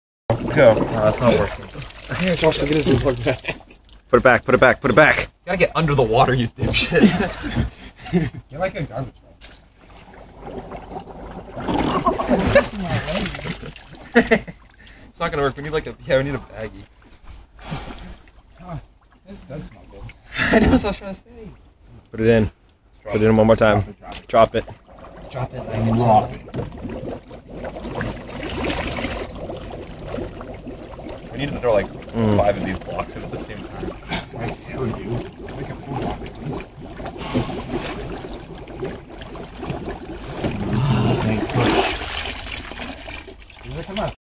Dry ice in a hot tub
This is what happens on my brothers 18th birthday